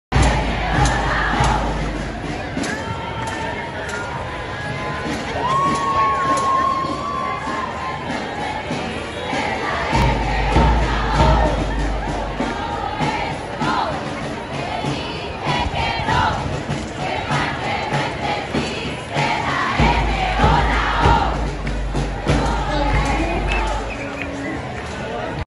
Día Internacional de la No violencia hacia las mujeres.
Miles de mujeres salieron a protestar en las calles de Costa Rica, en contra de la violencia de las mujeres